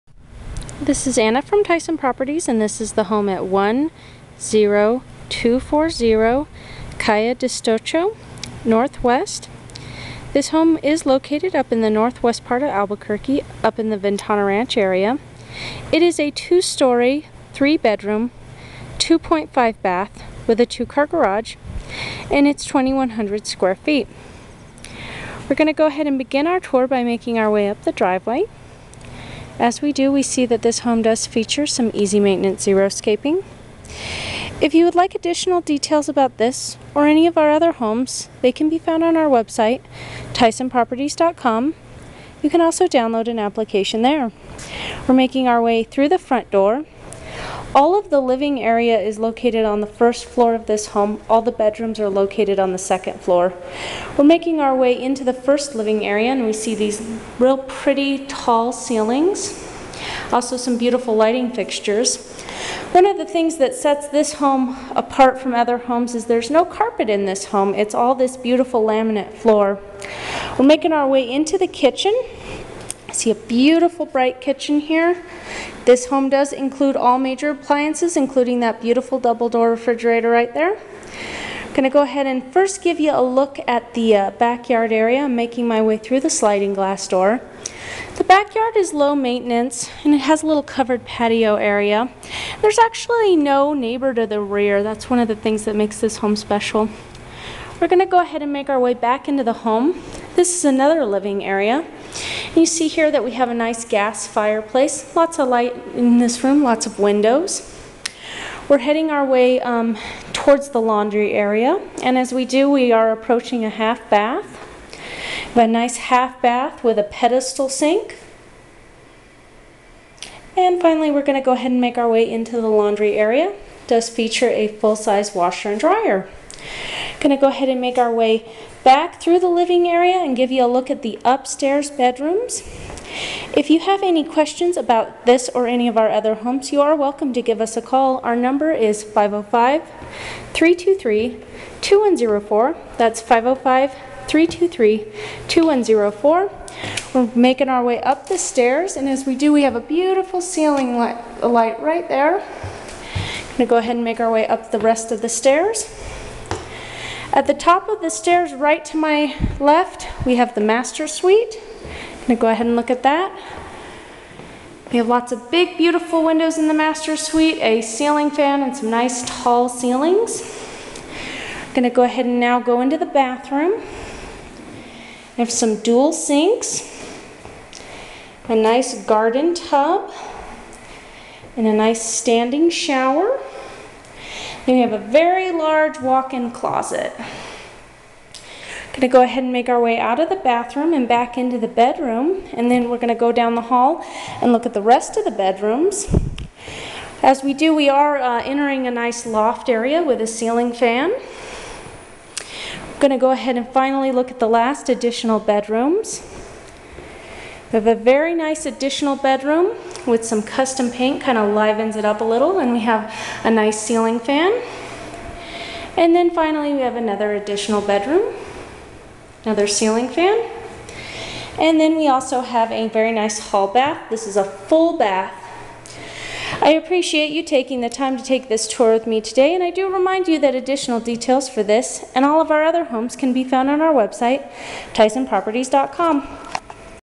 10240 Calle Dichoso -Lovely large home in Ventana Ranch!
Lovely large home in Ventana Ranch!  Beautiful laminate flooring throughout. No carpet. Two large living areas downstairs.  Gas fireplace.  All major appliances included.  High ceilings with lots of bright natural light.  Custom paint throughout.  Th